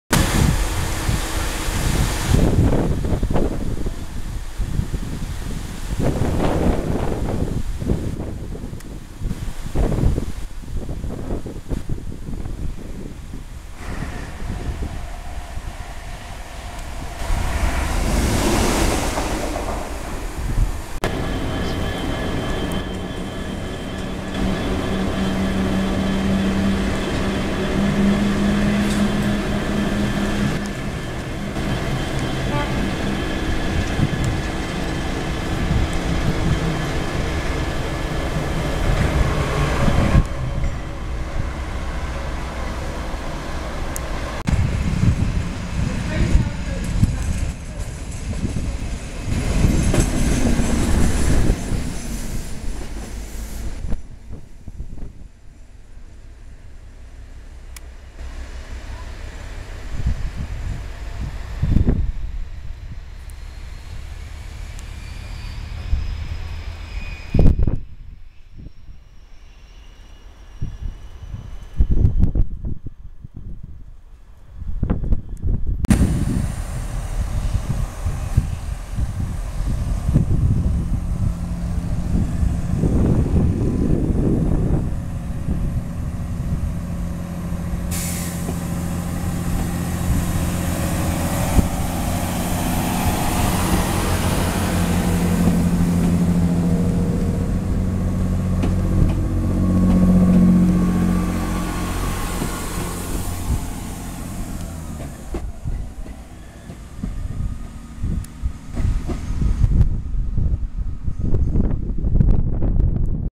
Special Movements at Didcot 29/05/2015
Special/rare movements at or around Didcot on the 29th May. Features Didcot Railway Centre's resident shunter, a Colas track machine and 56101 with a Chiltern MK3!